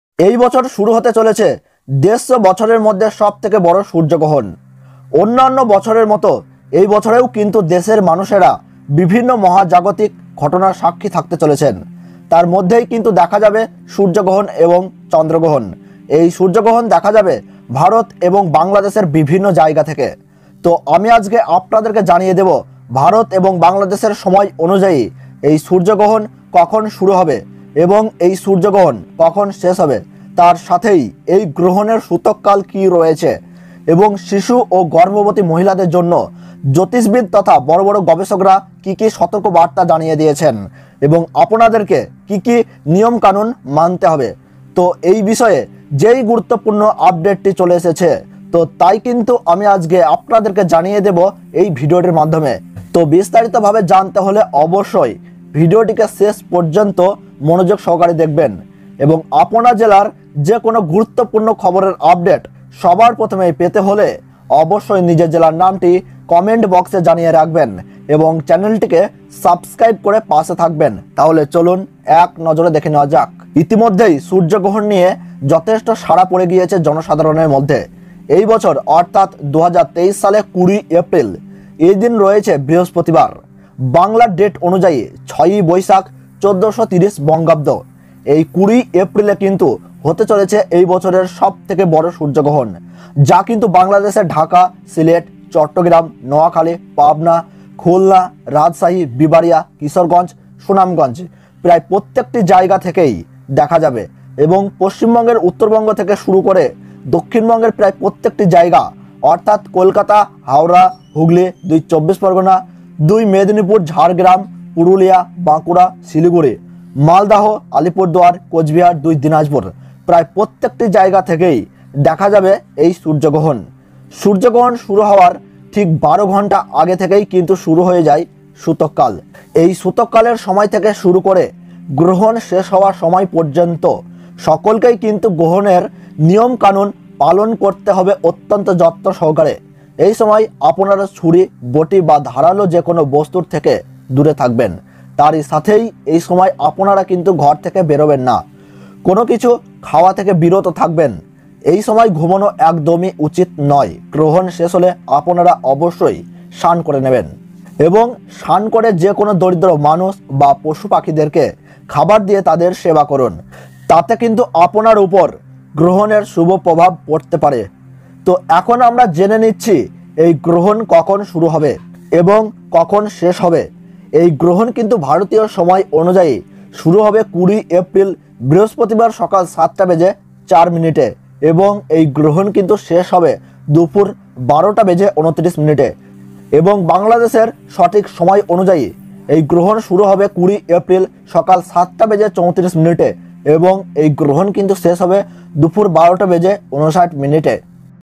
কাল বিগত ১৫০ বছরের মধ্যে সব থেকে বড় সূর্যগ্রহণ হতে চলেছে || Surya Grahan 2023 || Solar Eclipse 2023
কাল বিগত ১৫০ বছরের মধ্যে সব থেকে বড় সূর্যগ্রহণ হতে চলেছে || Surya Grahan 2023 || Weather News

surya grahan 2023 solar eclipse 2023 surya grahan 20 april 2023 surya grahan kab hai